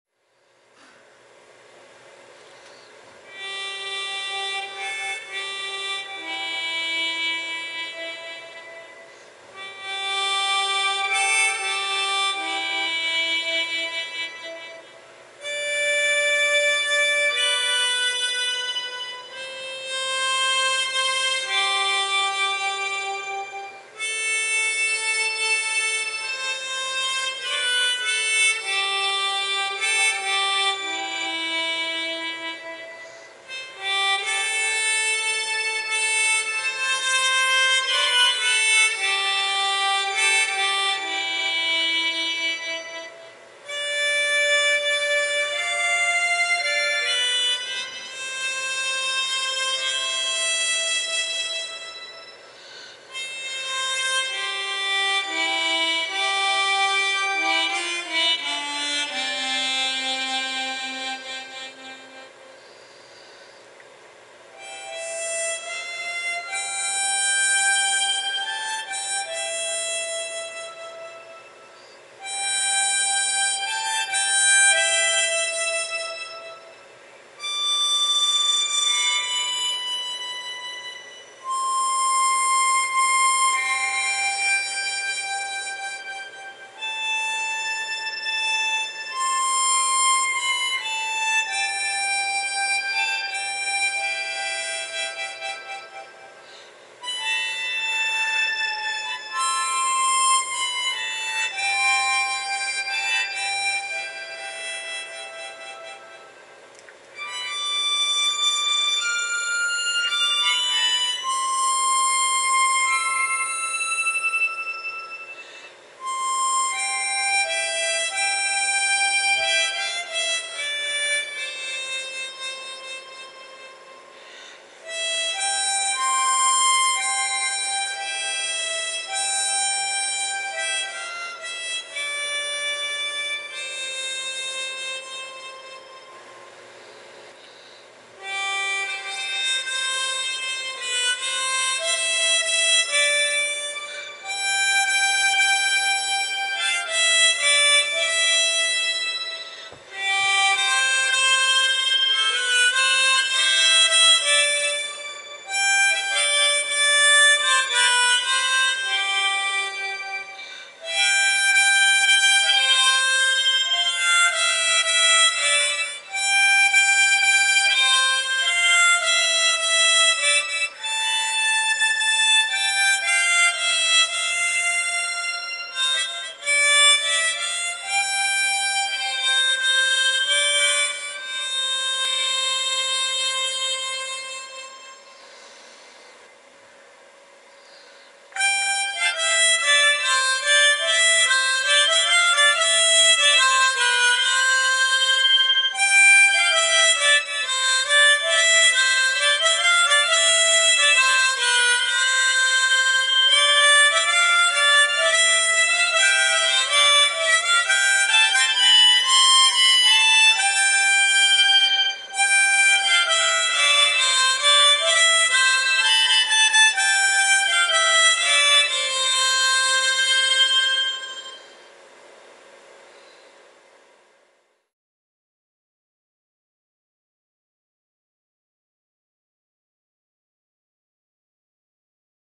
12 18 2023 A Short Christmas Medley
Playing a Swan 1040 Chromatic harmonica.
I am not the best player but I hope you enjoy.